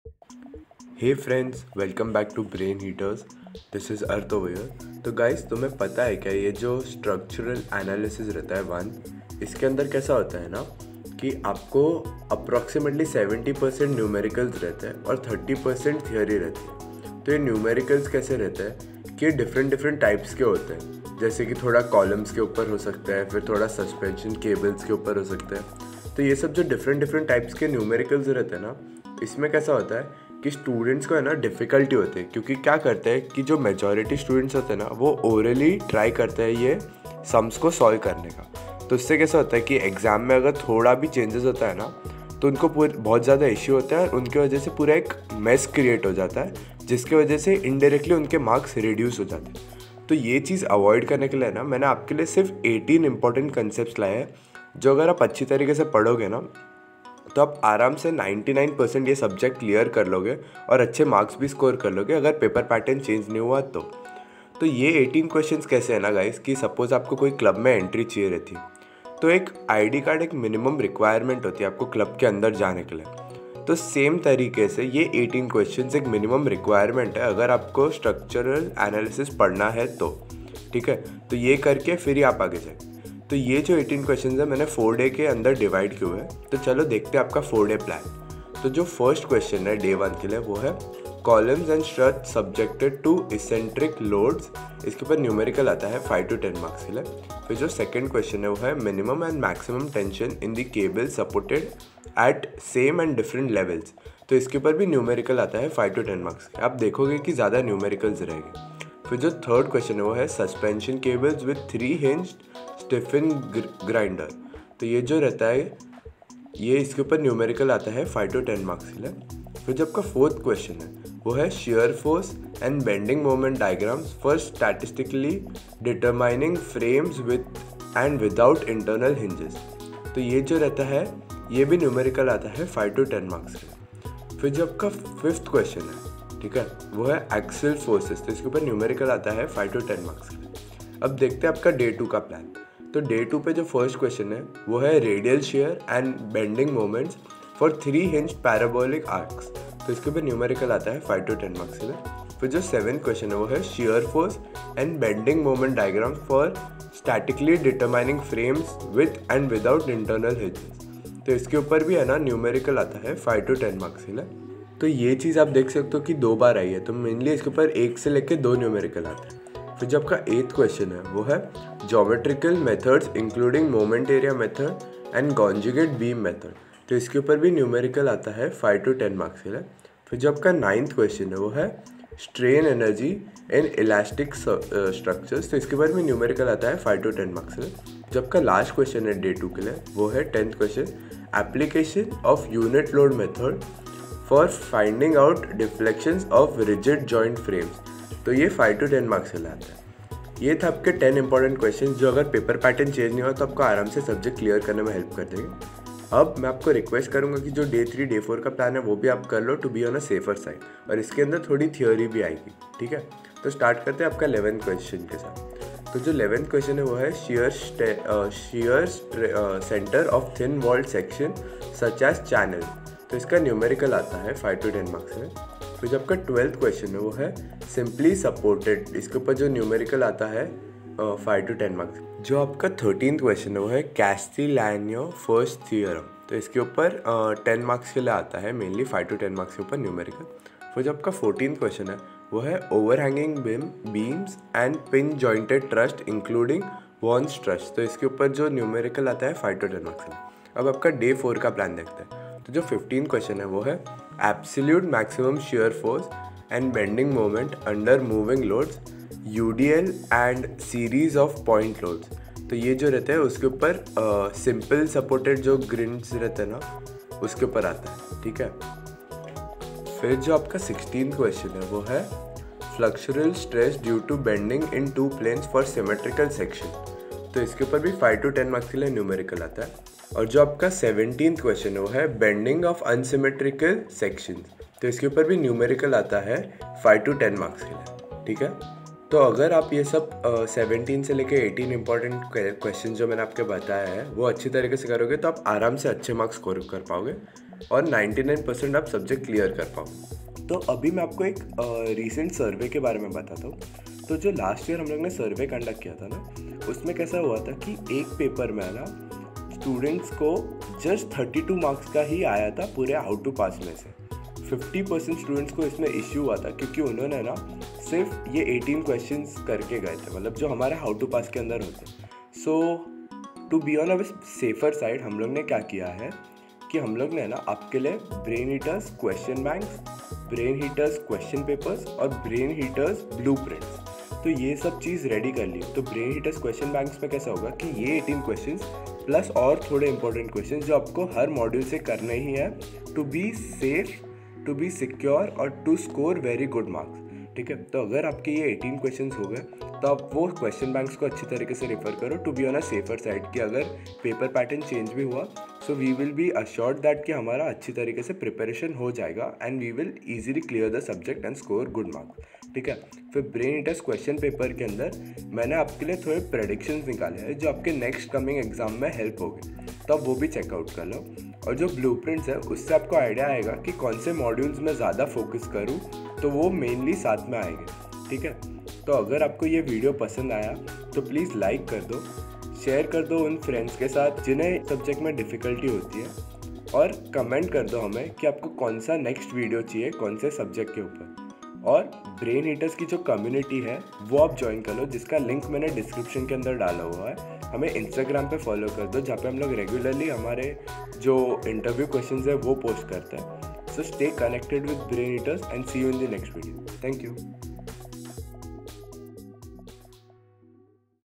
How to Pass/Score SA-1(Structural Analysis-1) in 3-4 days | Sem 4 Civil
Hey Smart Engineers,
In this video, I am going to show you How to Pass  SA-1(Structural Analysis-1) in 3-4 days. This video contains all the important concepts for SA-1.